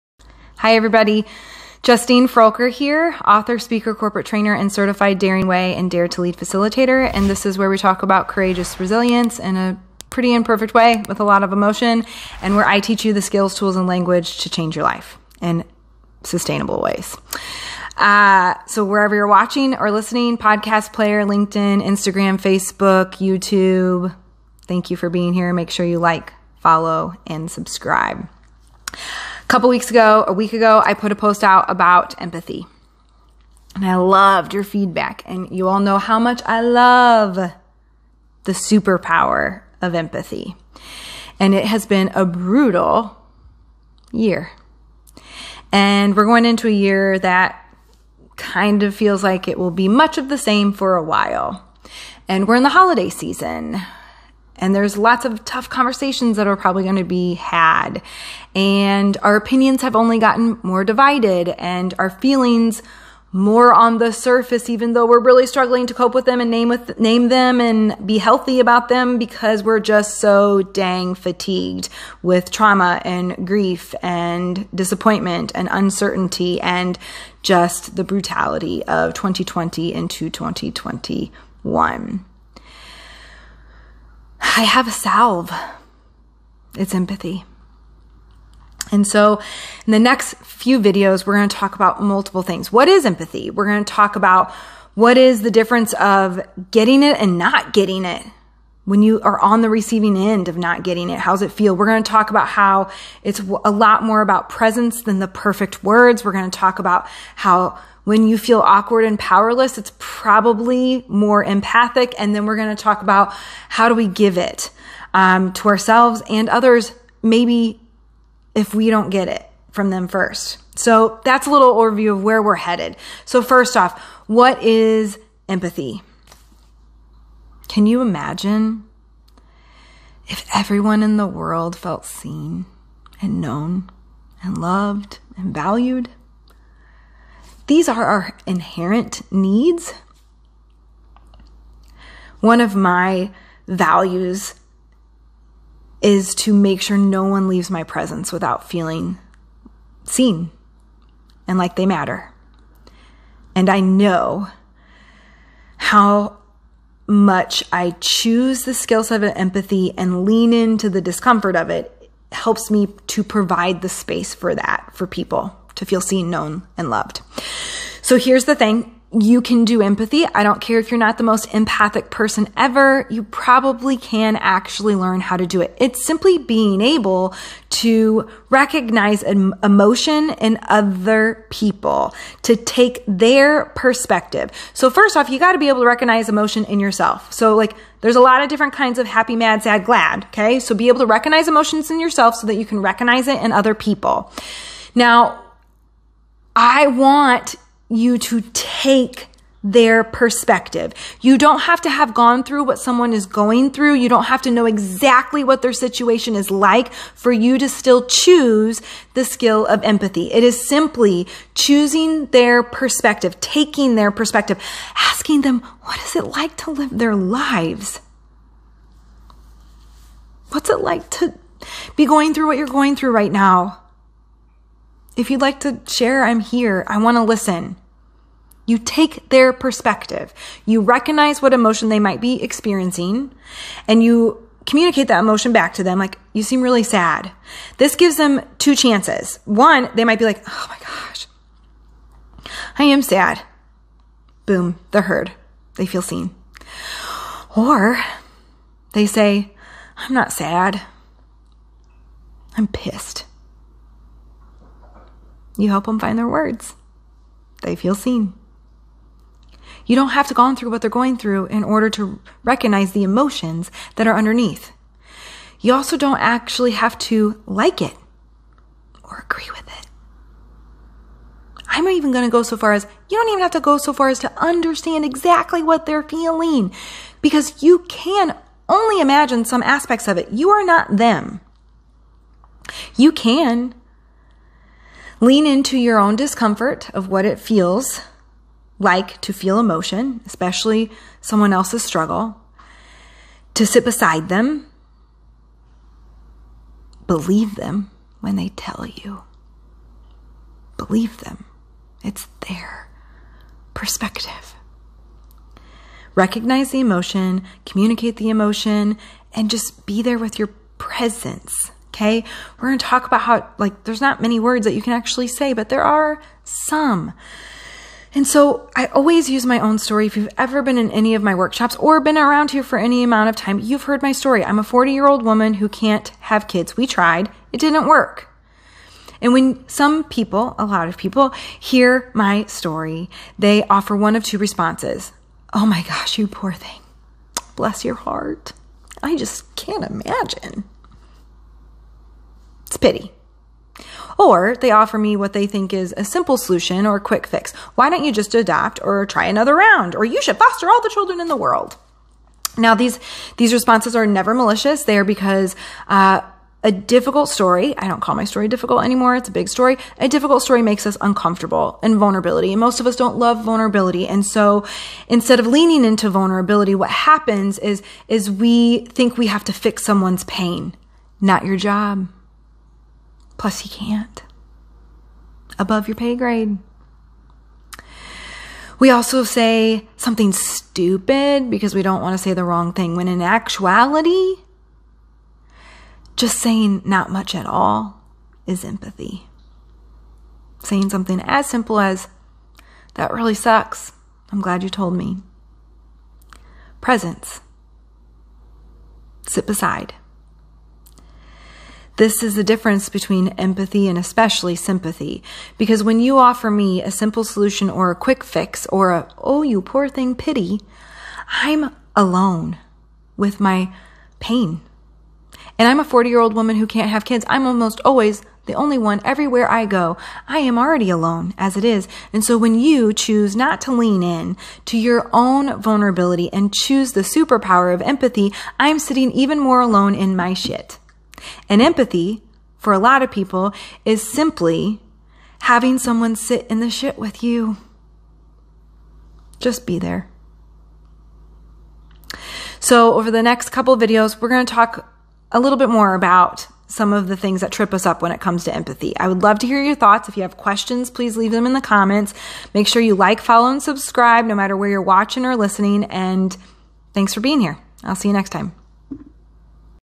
What is Empathy? And How It Helps in 2020!
Justine Froelker is a Bestselling Author, Speaker, and Certified Daring Way™ & Dare to Lead™ Facilitator (based on the research of Brené Brown) begins an empathy series based on your questions. Today we talk about what empathy is and what it's not. Join the conversation in the comments!

Justine's latest book

Justine’s Awesome T-Shirts